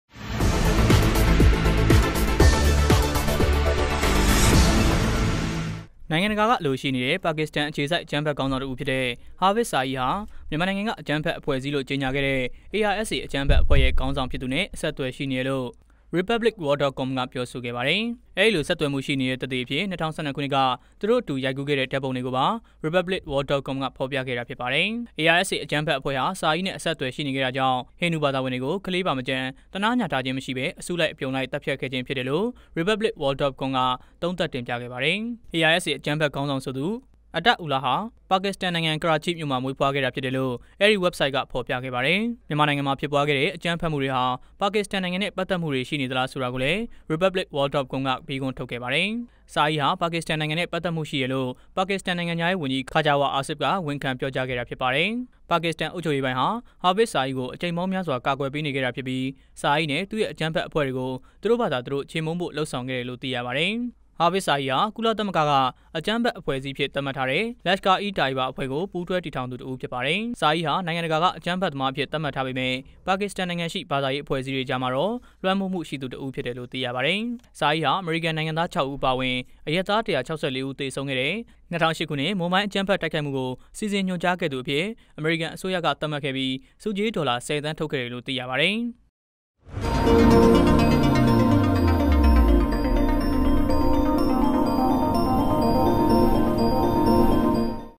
ႏိုင္ငံတကာအလိုရွိေနသည့္ ပါကစၥတန္အေျခစိုက္အၾကမ္းဖက္ေခါင္းေဆာင္တစ္ဦးႏွင့္ ARSA အၾကမ္းဖက္
ႏိုင္ငံတကာကအလုိရွိေနတဲ့ ပါကစၥတန္အေျခစိုက္ အၾကမ္းဖက္ေခါင္းေဆာင္တစ္ဦးျဖစ္တဲ့ ဟာဖစ္ဇ္ ဆာအိဟာျမန္မာႏုိင္ငံကအၾကမ္းဖက္အဖြဲ႔အစည္းလို႔ ေၾကညာခဲ့တဲ့ ARSA အၾကမ္းဖက္အဖြဲ႔ရဲ႕ ေခါင္းေဆာင္ျဖစ္သူ နဲ႔ အဆက္အသြယ္ရွိေနတယ္လို႔ republicworld.com က ေျပာဆိုခဲ့ပါတယ္။